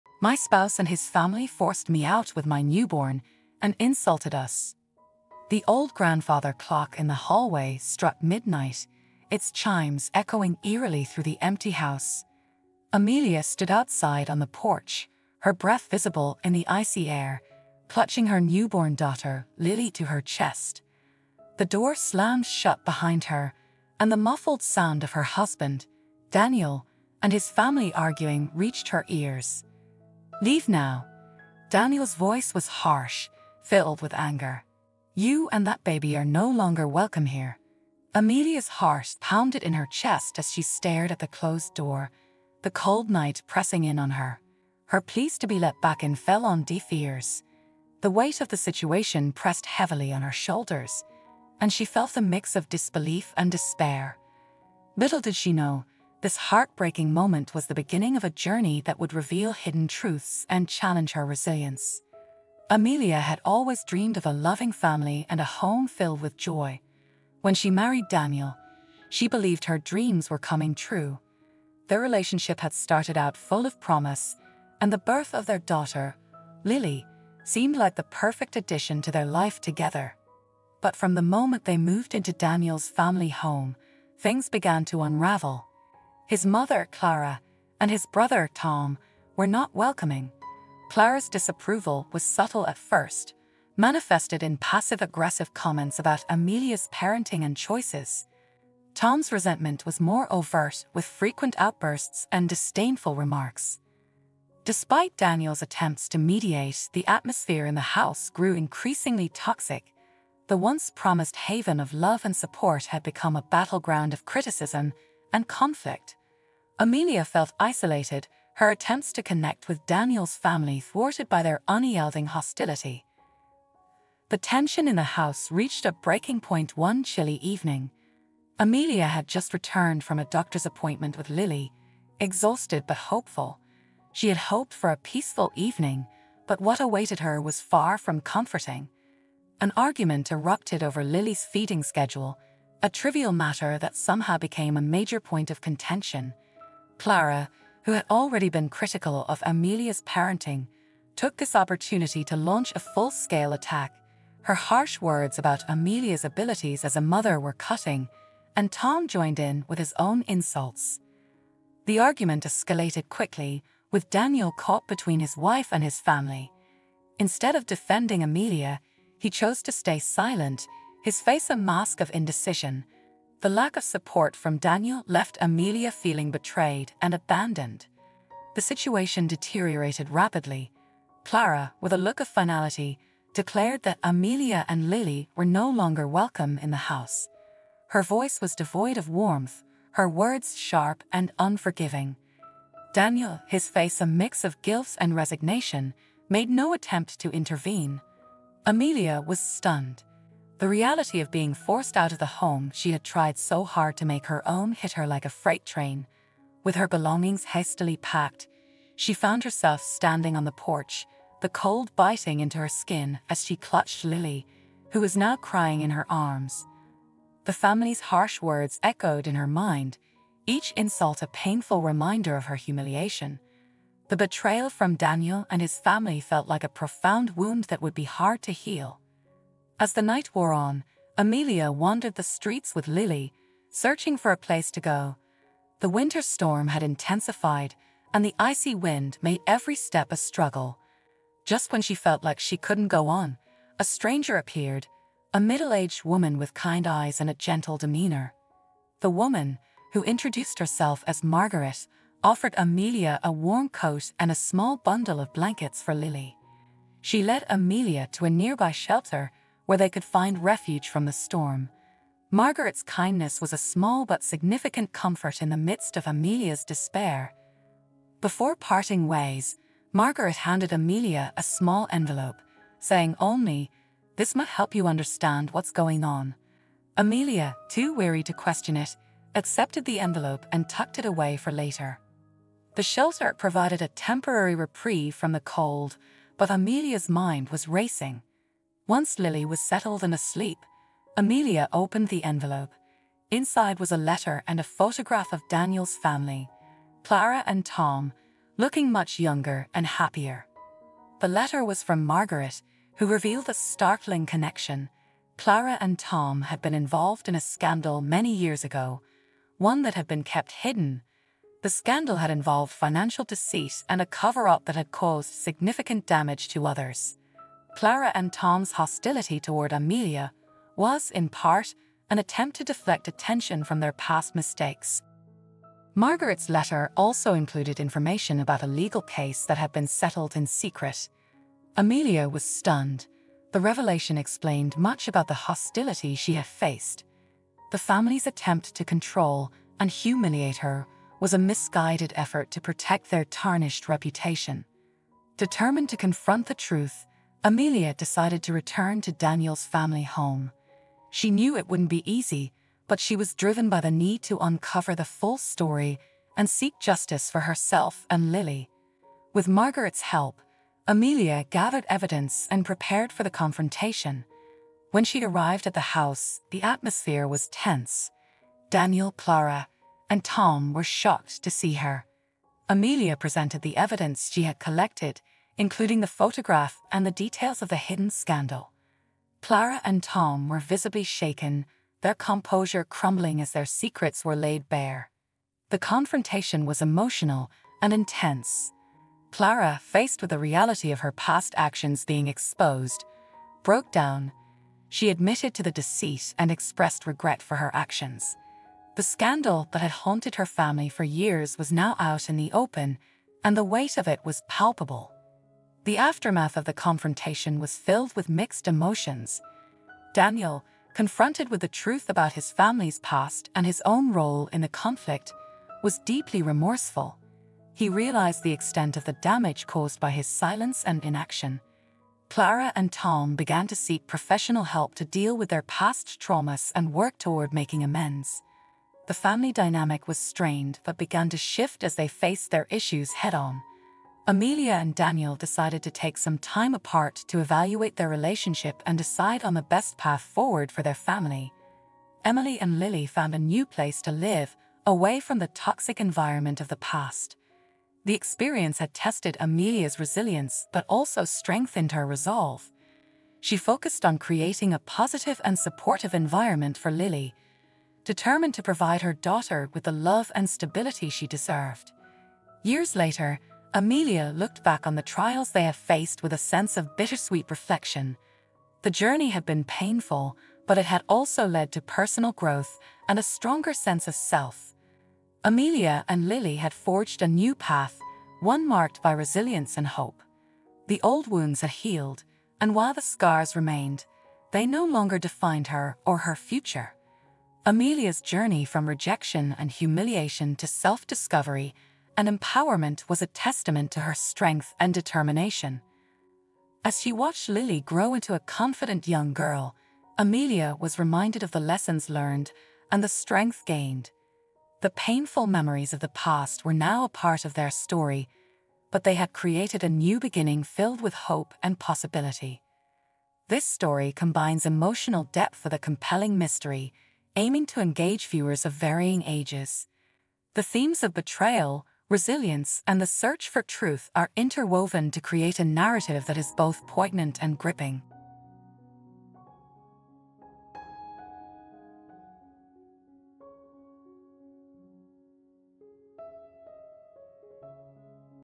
My Spouse and His Family Forced Me Out with My Newborn and Insulted Us
The old grandfather clock in the hallway struck midnight, its chimes echoing eerily through the empty house. Amelia stood outside on the porch, her breath visible in the icy air, clutching her newborn daughter, Lily, to her chest. The door slammed shut behind her, and the muffled sound of her husband, Daniel, and his family arguing reached her ears